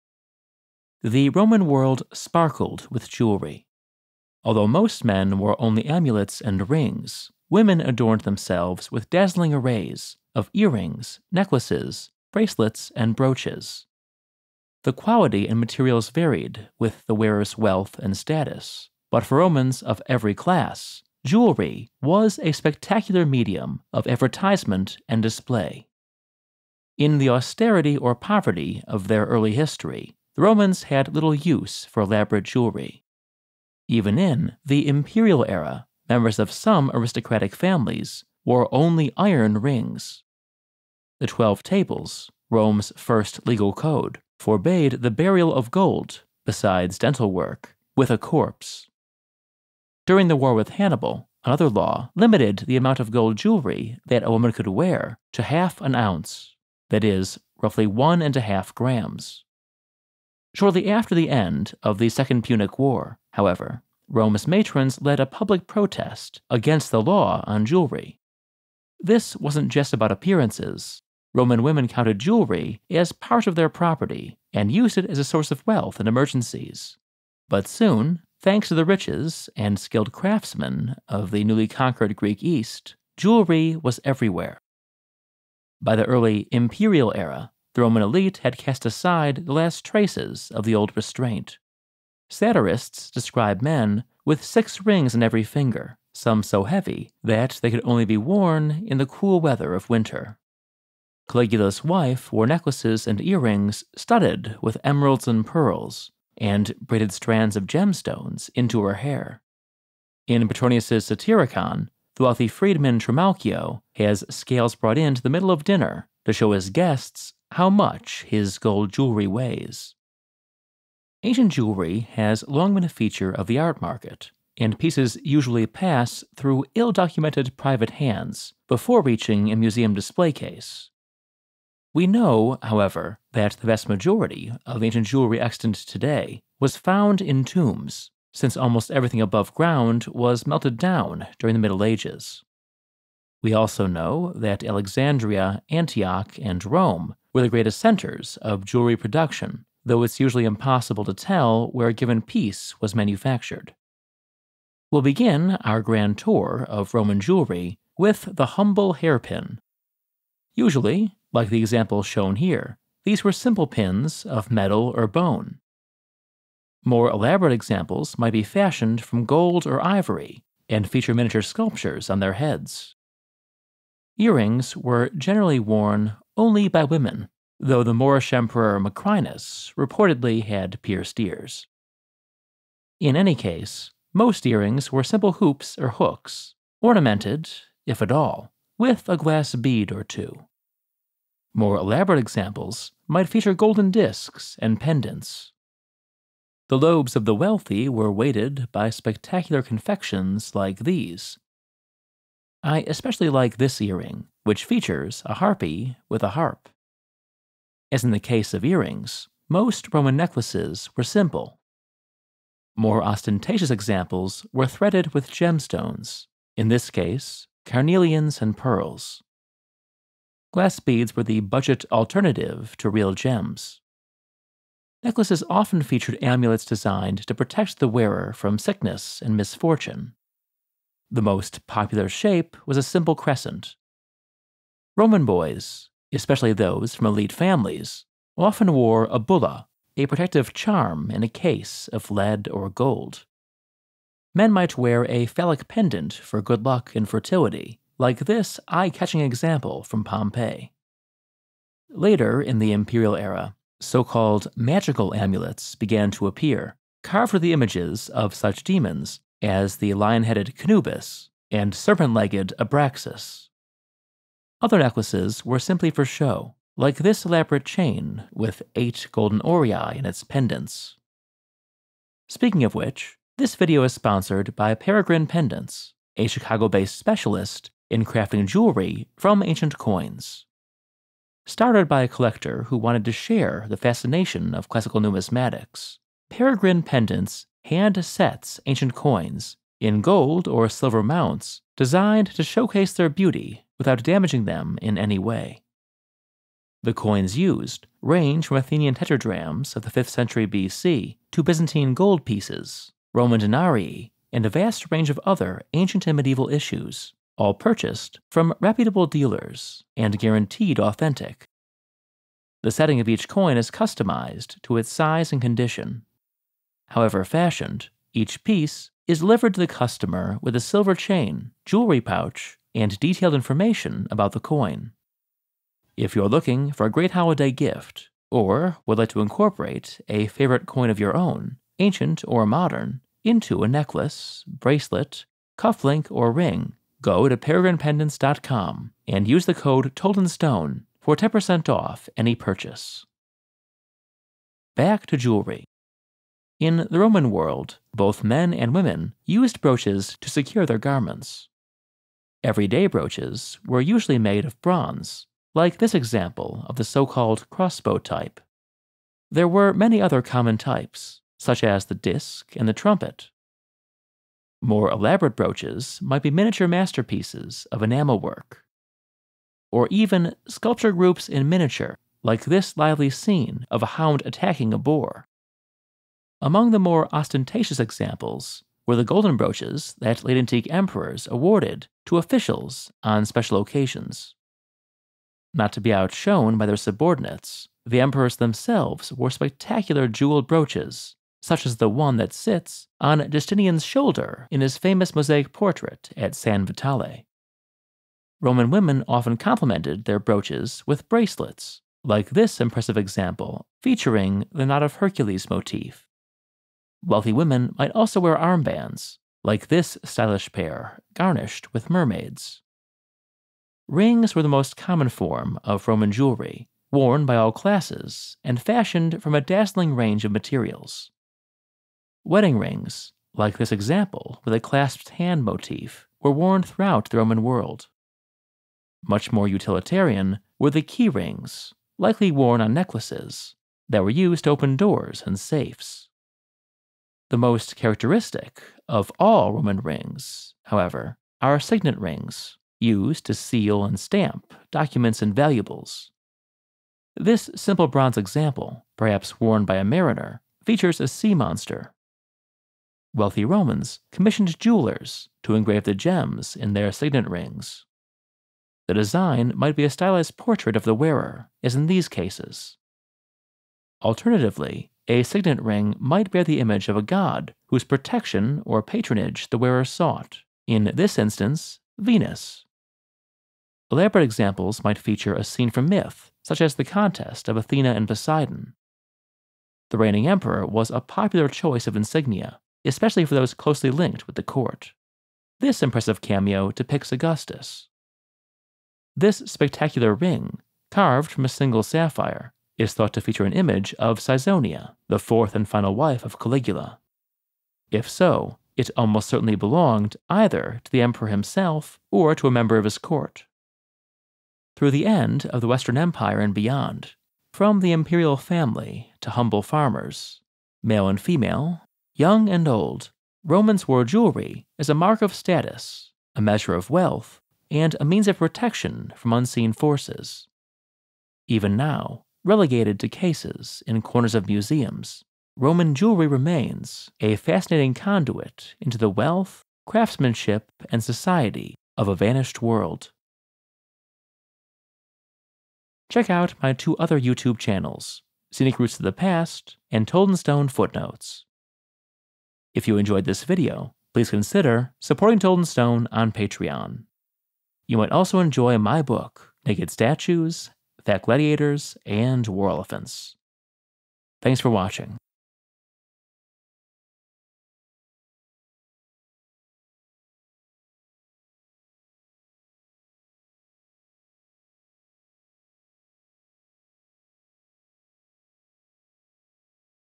Rings and Jewelry in Ancient Rome
The Roman world sparkled with jewelry. The quality and materials varied with the wearer’s wealth and status. But for Romans of every class, jewelry was a spectacular medium of advertisement and display.

If you liked this video, you might also enjoy my book “Naked Statues, Fat Gladiators, and War Elephants: Frequently Asked Questions about the Ancient Greeks and Romans.”

Chapters:
0:00 Introduction
0:27 A brief history of Roman jewelry
2:01 Problems of provenance
2:35 Hairpins and earrings
3:28 Necklaces and amulets
4:32 Peregrine Pendants
5:44 Brooches and bracelets
6:54 Rings
8:24 Final thoughts